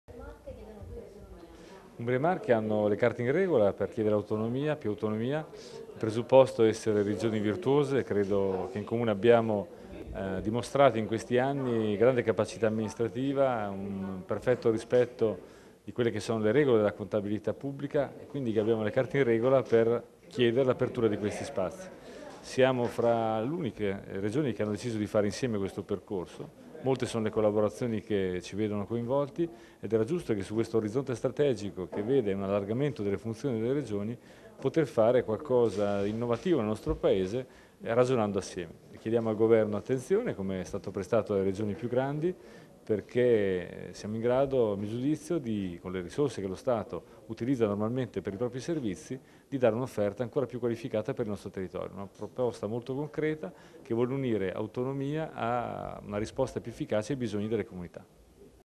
Ceriscioli (Marche) con Umbria abbiamo carte in regola per chiedere più autonomia
Incontro fra Regioni Marche e Umbria per l’avvio di percorso previsto dal terzo comma dell'art.116 della Costituzione individuando le ulteriori forme e condizioni di autonomia per chiedere l'attivazione dello specifico tavolo di confronto istituzionale, a margine della Conferenza delle Regioni e delle Province Autonome di oggi